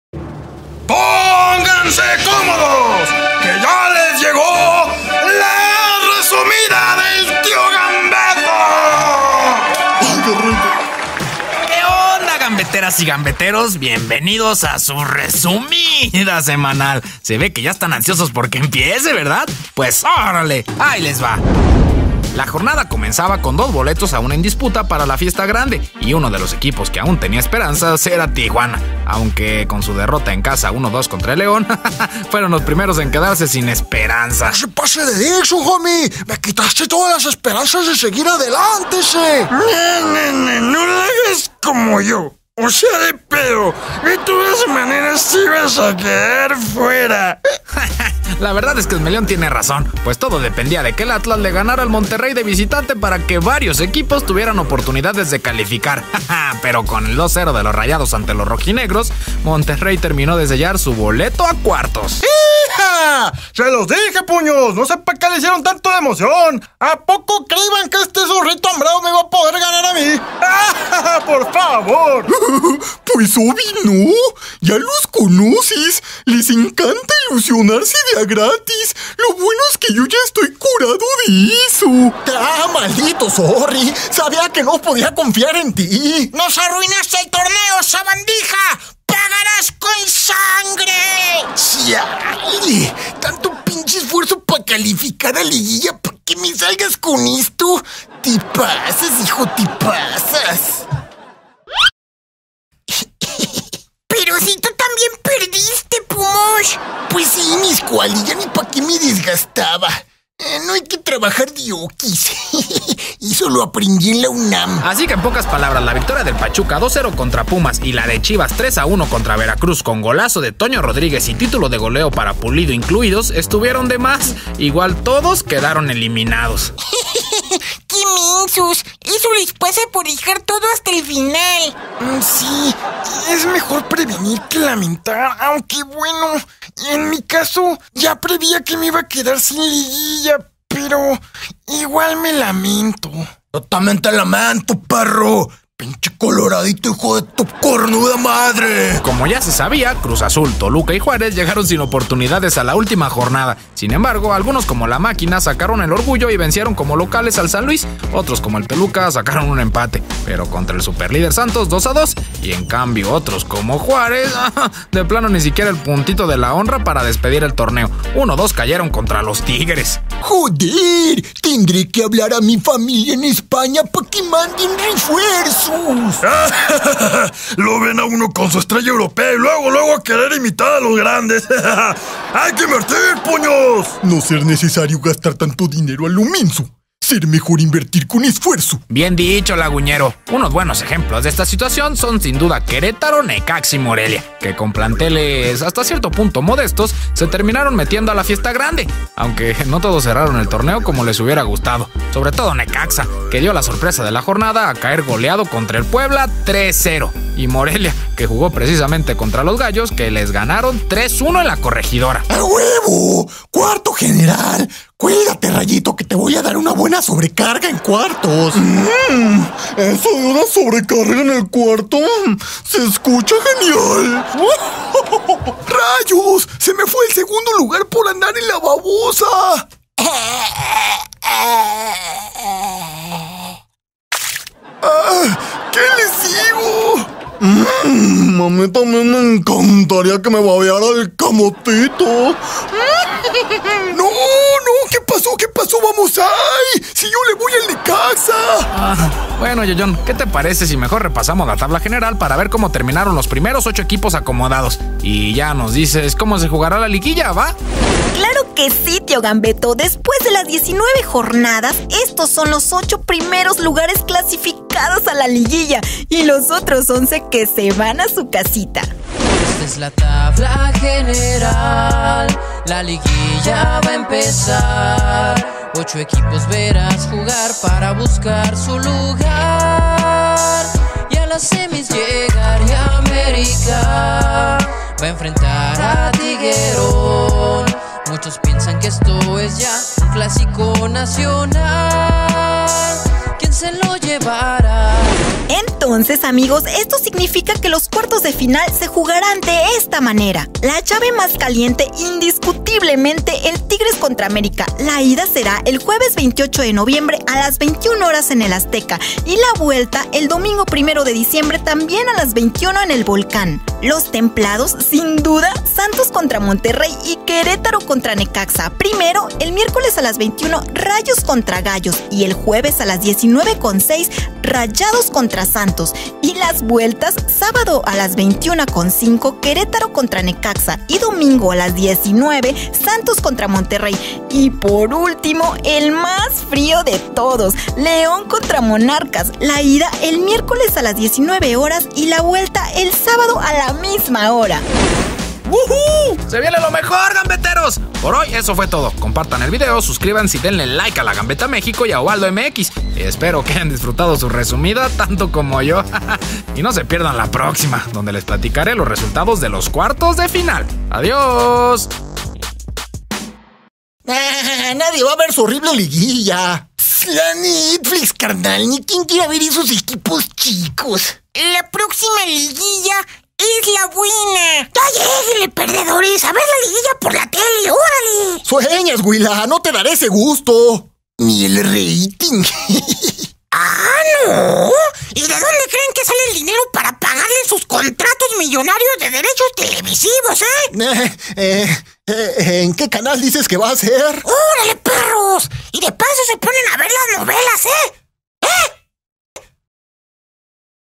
¡Chivas para AFUERA! Y Rayados se Cuela! | La Resumida
¿Creyeron que no se las íbamos a resumir hoy? 🤔¡Pues toma que sí! 😎

La liga regular terminó😮⚽️, pero en la liguilla tendremos verdaderos duelazos🔥, ¿Tigres América clásico? 🤔